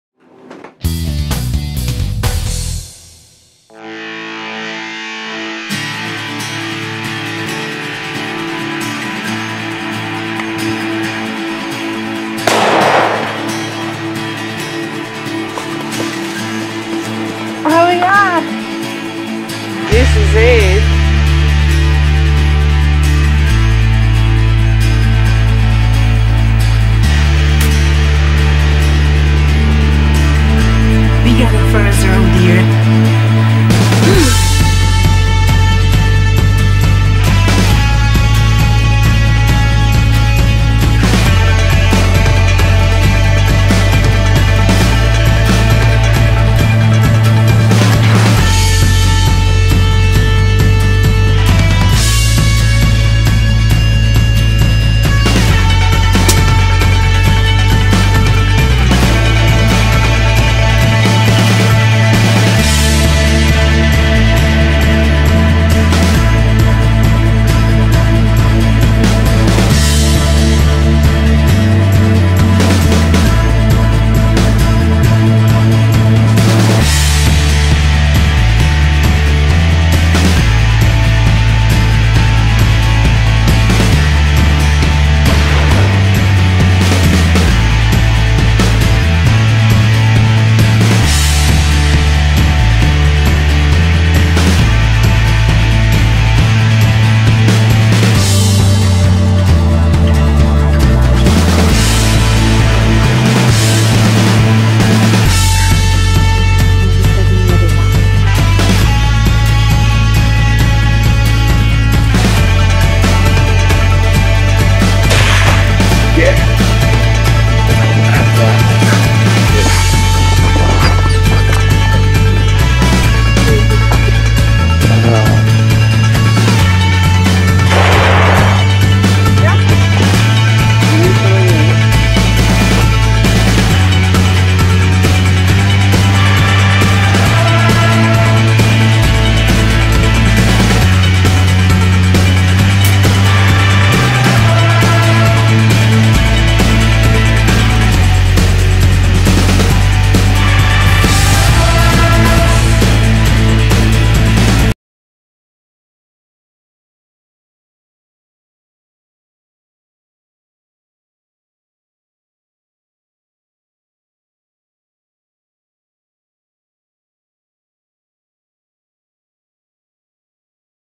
Michaelka's Hunting European highlights
Michaela and her hunting adventure highlights. - Czech,Slovakia,Hungary,Sweden,England